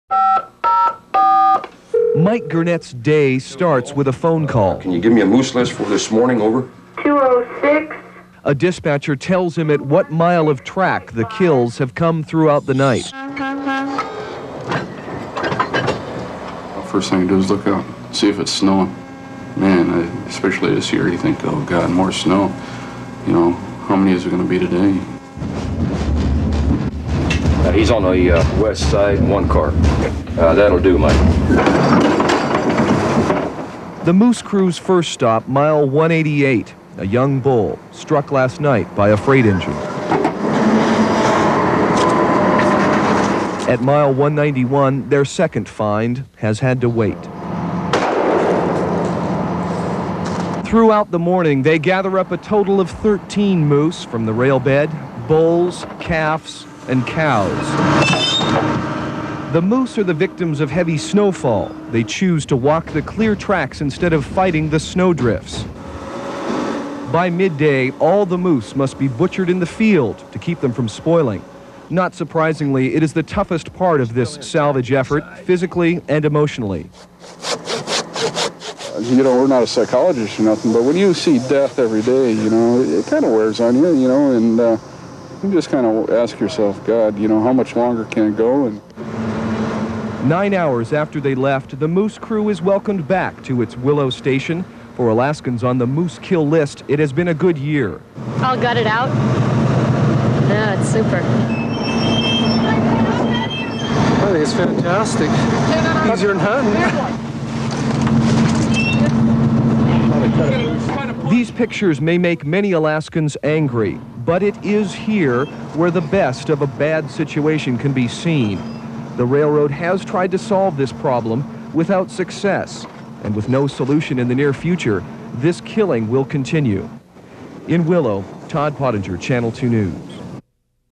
MooseSalvage.mov
*Not for the faint-hearted*.  Alaska Railroad trains hit and kill hundreds of moose every year.  They don't go to waste. The reporter is Todd Pottinger, the cameraman/editor is John Ford.  This story originally aired in 1989 on KTUU Anchorage.